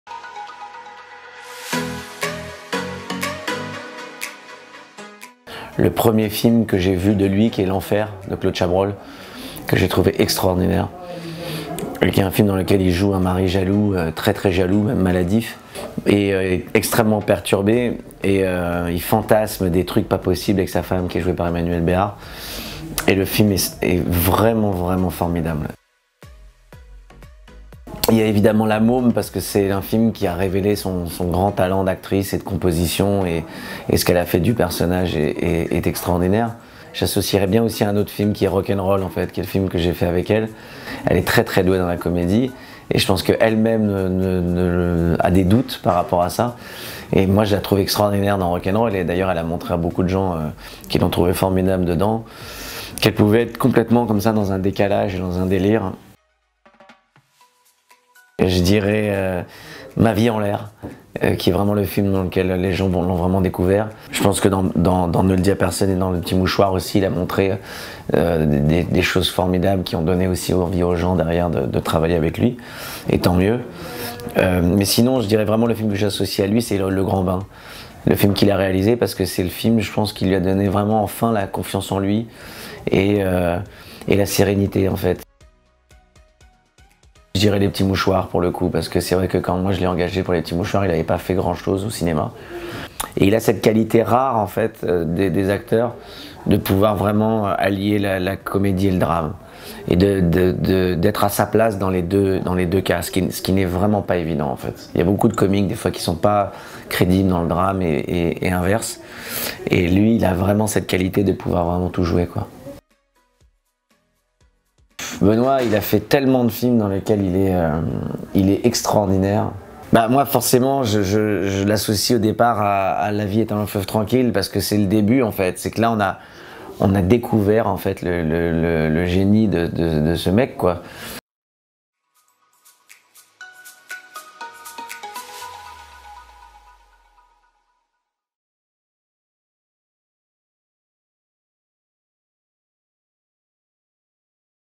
Guillaume Canet - ITW Un ami Un film
We Love Cinema a demandé à Guillaume Canet d'associer les acteurs de Nous Finirons Ensemble à un film.

Retrouvez We Love Cinema !